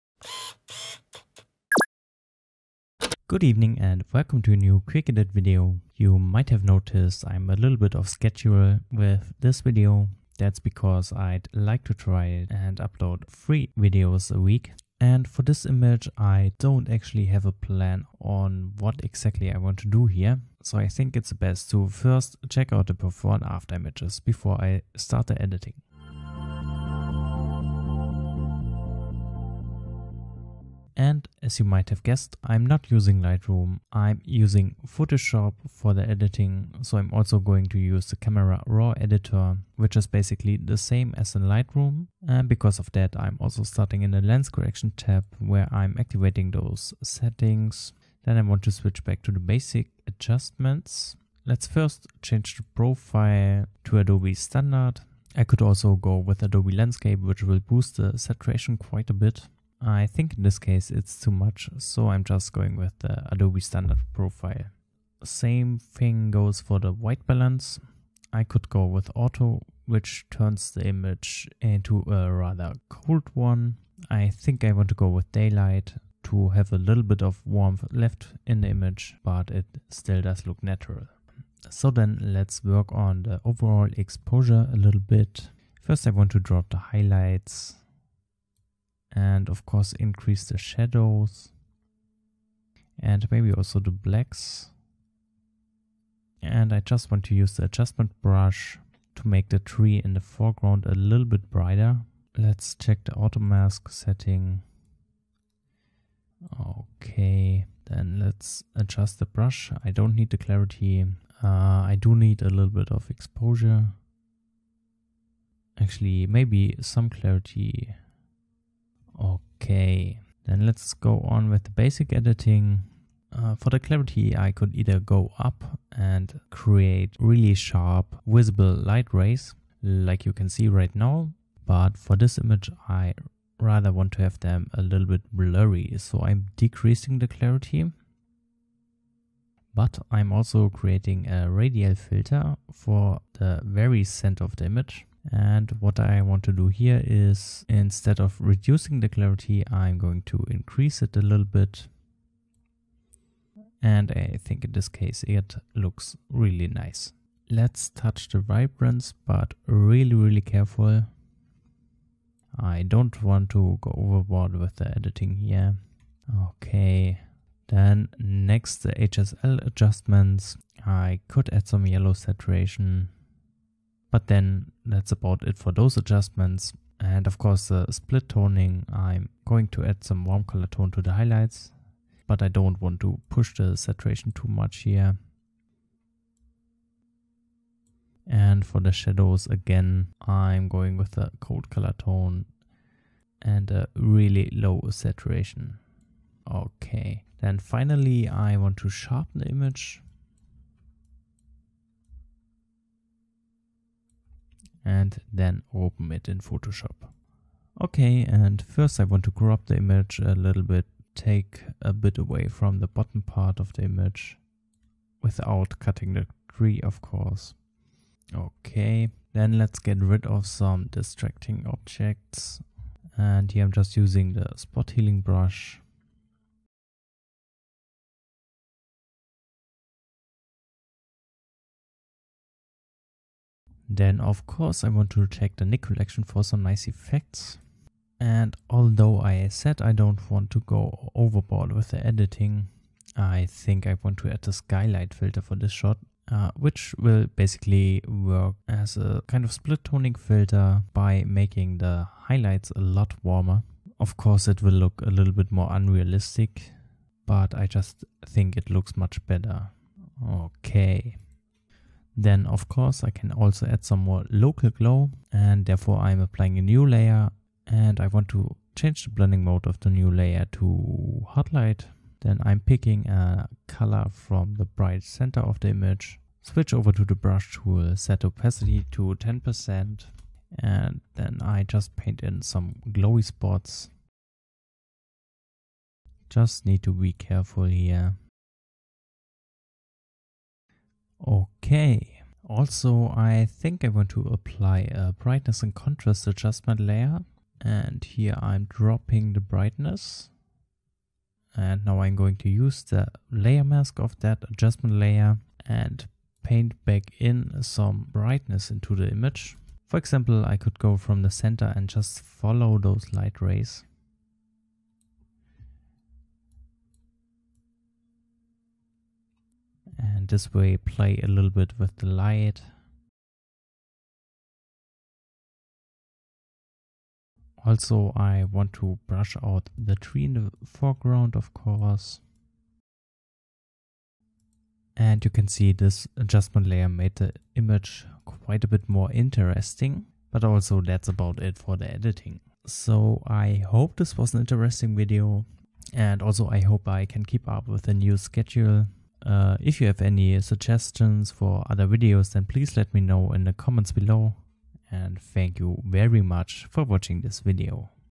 Subtle Sunray Editing with Photoshop | QE #167
In this quickedit video I'm applying some subtle PostProcessing to bring out beautiful warm colortones of a forest landscape. for the editing I will be using Adobe Photoshop (with it's Camera Raw editior).

So first I had to do the basic Raw adjustments. Here I changed the used color profile to reduce the saturation a little bit and bring back details from the darker areas of the photo. Also I changed the white balance to further redcude the saturation of those warmer, yellow tones and get a bit more natural light throughout the image. Of course the highlights and shadows needed to be adjusted so I dont have too much over- or underexposed areas (a little bit is ok in my opinion). Also I raised the blacks to brighten up the picture. Also very important here was to reduce the clarity which results in more blurry lightrays.

There was a little bit of color grading going on by increasing the vibrance and the saturation of the yellow tones. I also applied split toning to warmen up the highlights while applying a cold tone to the darker areas. Then finally I was done in the Raw editor and could apply some Nik Collection effects to further enhance the colors. With a hard light layer I painted in some subtle glowy spots throughout the picture. Finally I added a brightness / contrast adjustment layer to do some kind of dodging and burning (making certain areas brighter).